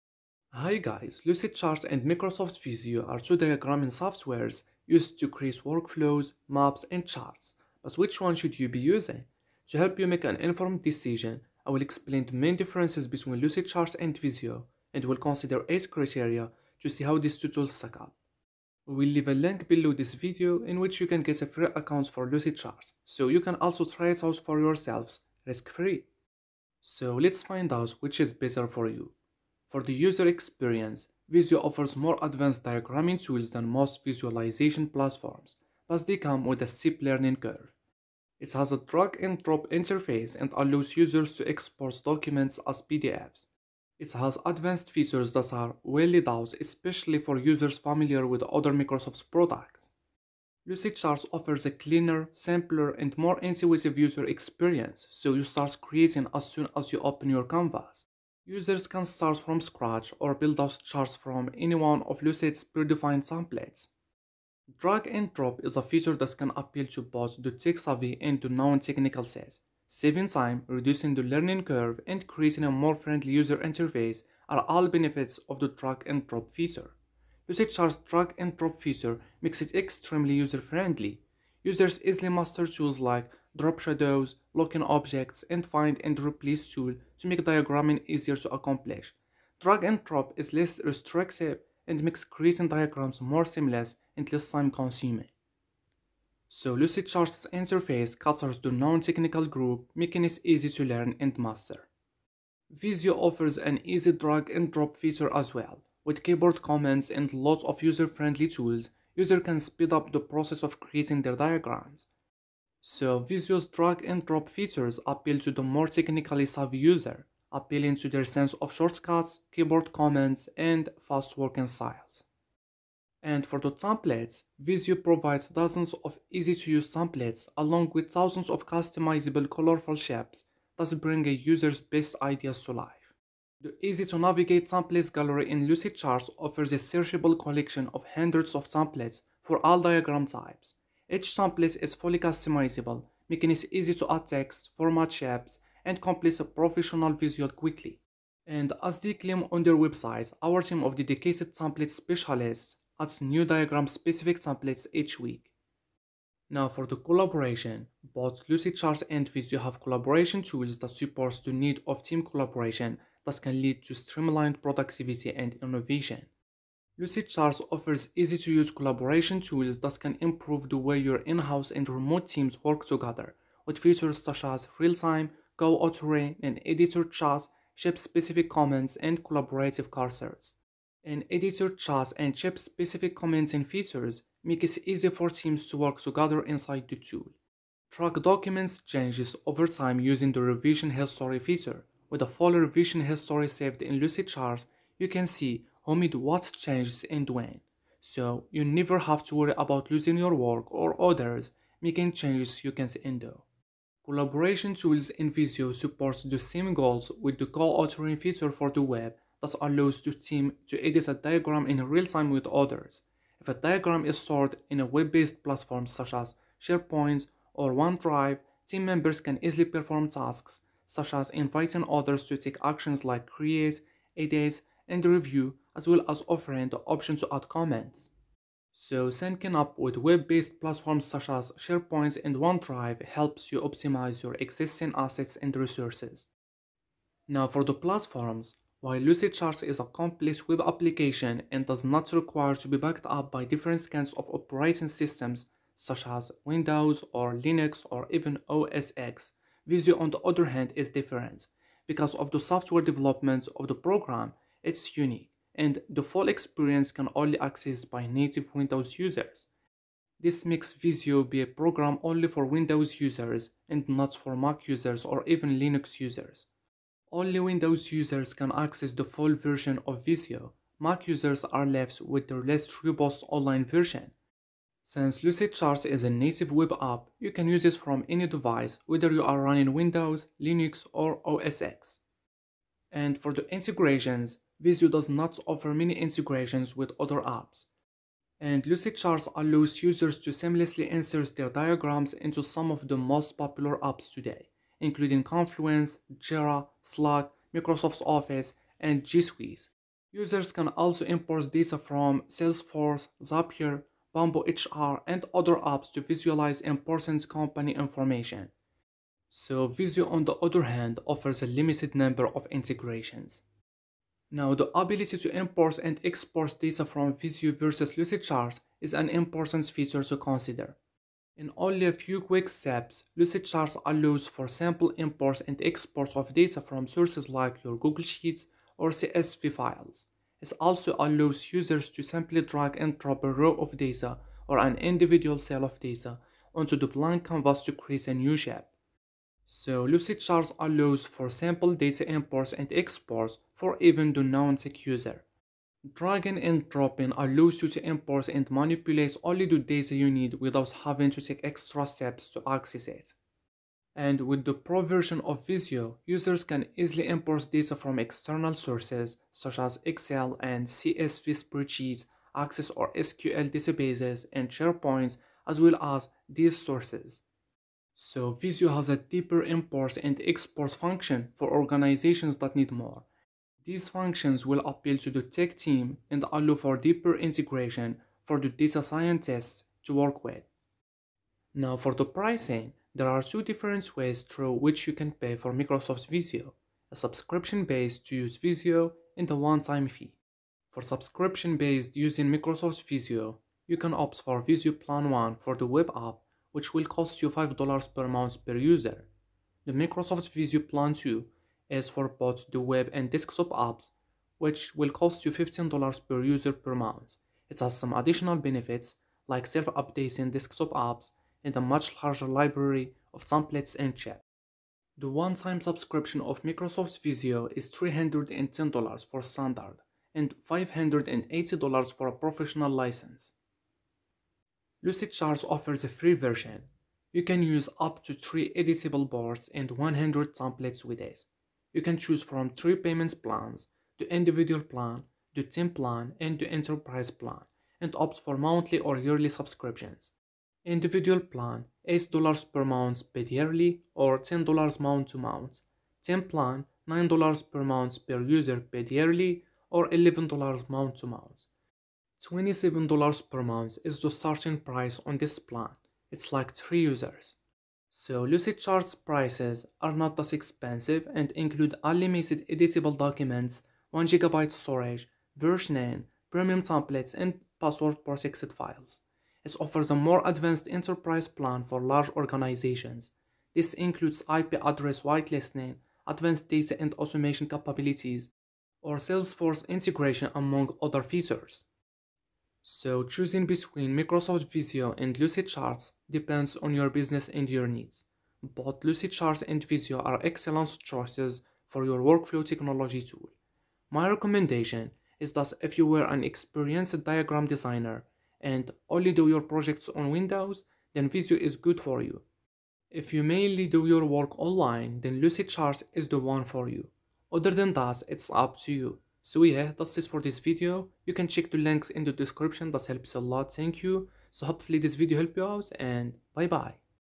Lucidchart vs Visio - Which One Is Better?
In this video you will find out Which One is Better - Microsoft Visio or Lucidchart

🔥 Special Offers 🔥

Chapters:
0:00 Intro
0:29 User Experience
1:04 Drag & Drop
2:02 Template
2:36 Collaborations
3:57 Platforms
4:42 Integrations
5:14 Data Import/Export
6:21 Pricing
8:02 Conclusion